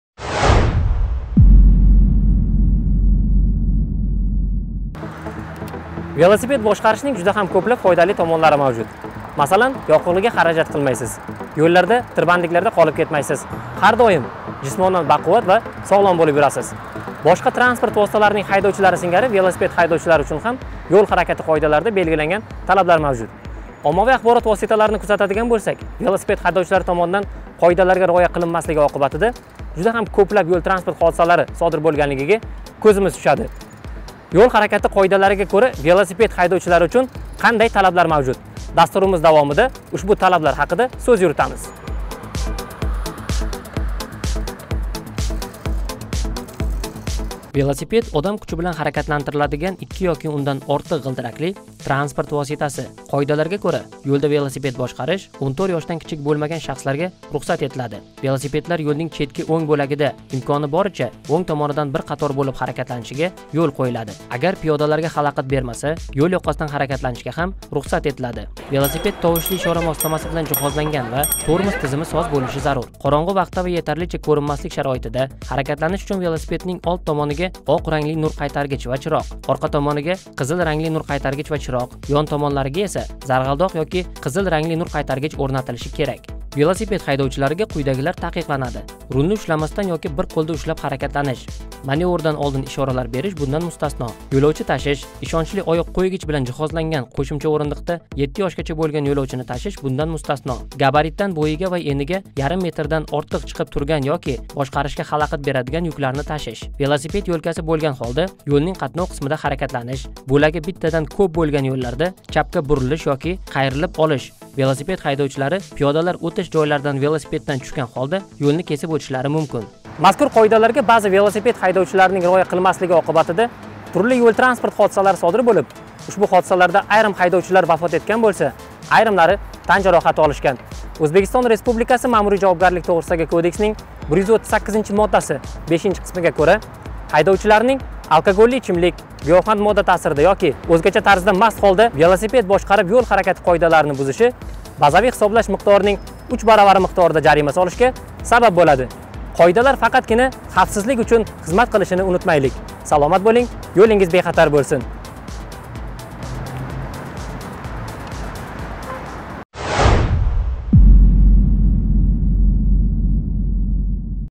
Velosiped boshqarishning ham o‘z tartib va talablari mavjud
Velosiped haydovchilarining yo‘l harakatini tartibga soluvchi signallarga bo‘ysunmasligi, ustunlik beruvchi, taqiqlovchi yoki ko‘rsatma beruvchi yo‘l belgilari talablariga rioya etmasligi - BHMning uchdan bir qismi (90.000 so‘m) miqdorida jarima solishga sabab bo‘ladi.

Velosiped haydovchisining alkogolli ichimlikdan, giyohvand modda ta’siridan yoki o‘zgacha tarzda mast bo‘lgan holda yo‘l harakati qoidalarini buzishi — BHMning uch baravari (810.000 so‘m) miqdorida jarima solishga sabab bo‘ladi.
Barcha haydovchilardan qoidalarga rioya qilishlarini so‘raymiz.